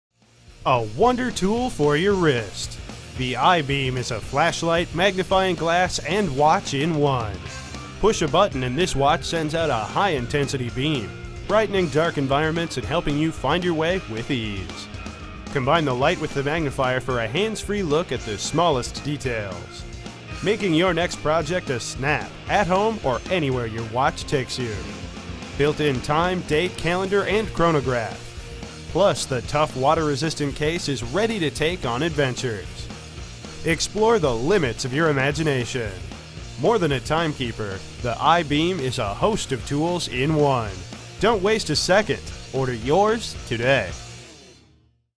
iBeam Digital Sport Flashlight Watch
Casts a 15 darkness-piercing light beam! This Digital Watch does more than display time. Its built in LED flashlight eliminates the need for an additional light source in most cases. Lets you tackle hands-free tasks while illuminating a large area.

Great for finding lost items. Even has a flip-up 3X magnifying lens to enlarge fine print on maps menus and documents for easier reading. Can be used with the sun to start a fire. Our timely buying power means LESS for you!